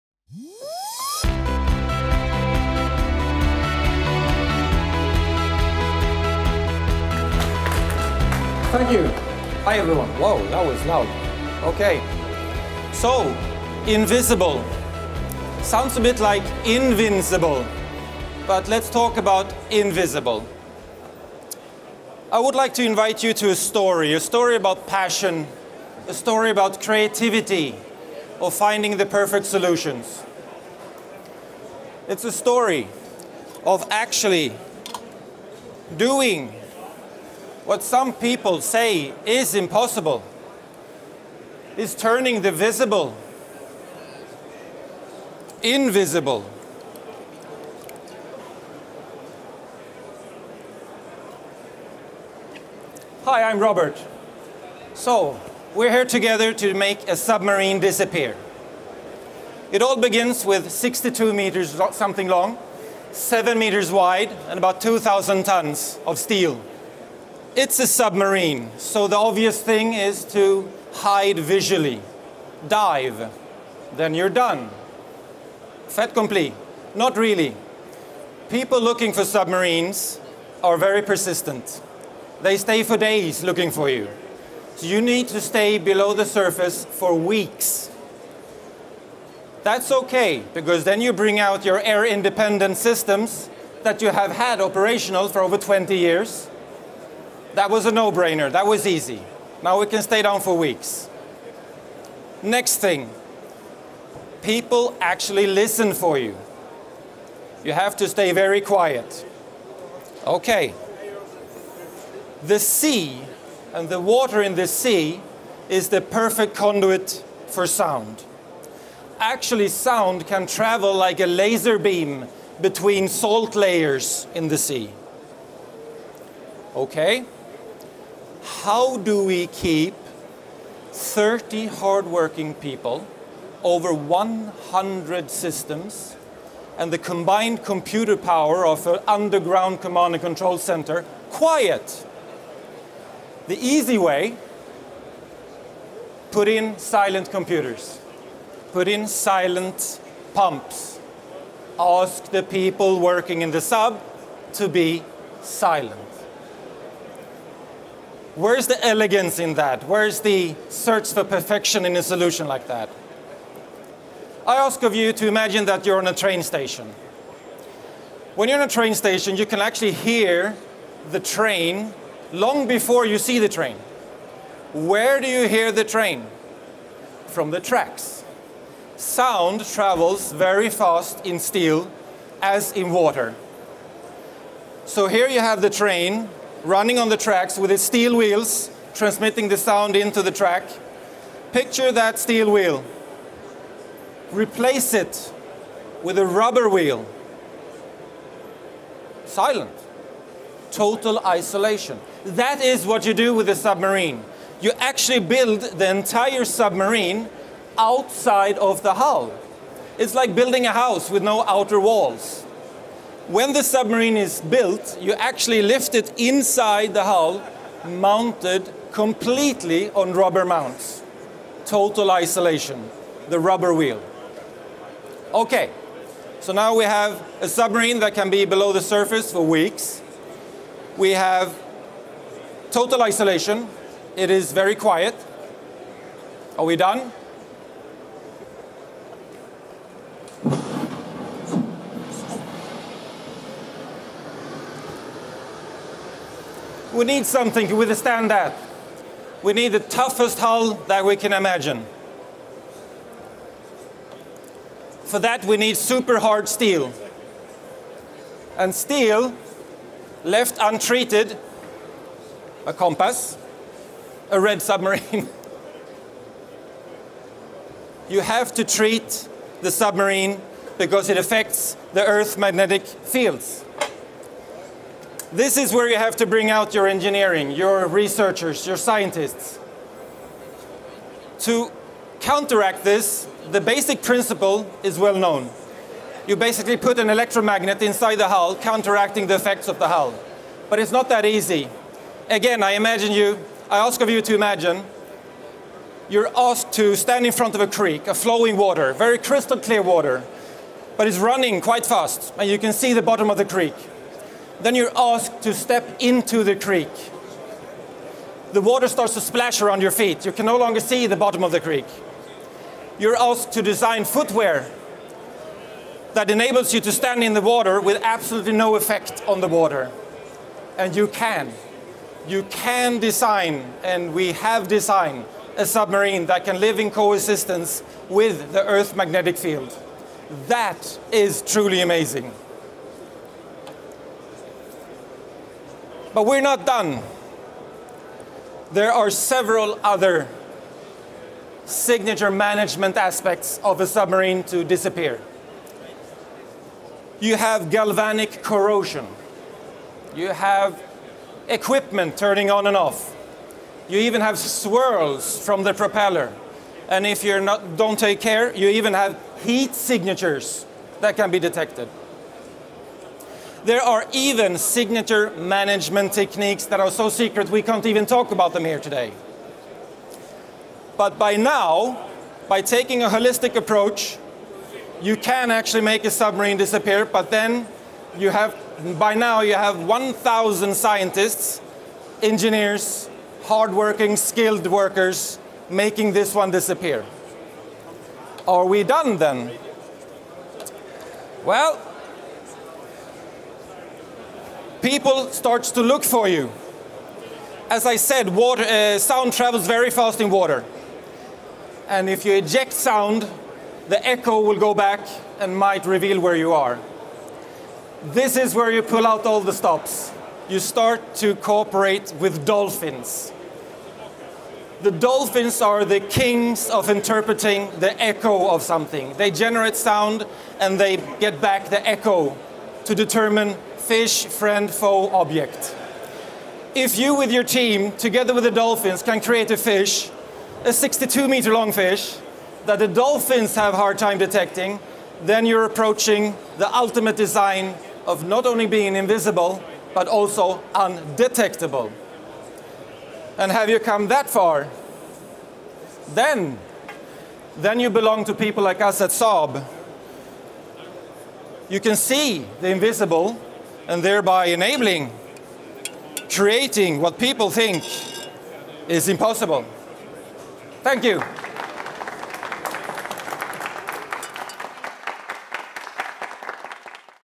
"How to make the visible, invisible" with Robert Edlund - Saab Minitalk
Saab Minitalk during Euronaval 2016 - Robert Edlund, Senior Sales Executive at Saab on A26 - How to make the visible, invisible